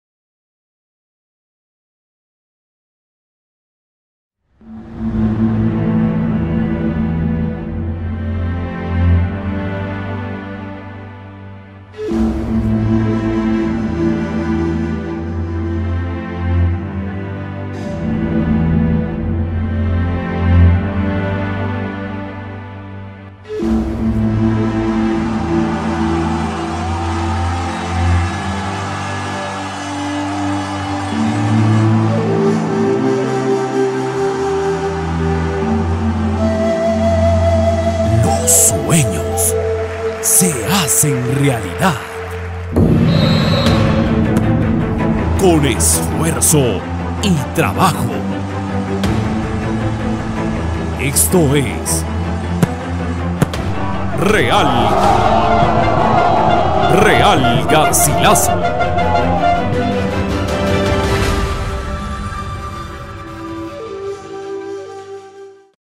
SPOT REAL GARCILASO - Qosqo Films
Cliente: Real Garcilaso
Proyecto: Campaña Promocional
Titulo: "Pequeños Sueños, Grandes Realidades"
Duracion: 1 min.
Produccion: QOSQO FILMS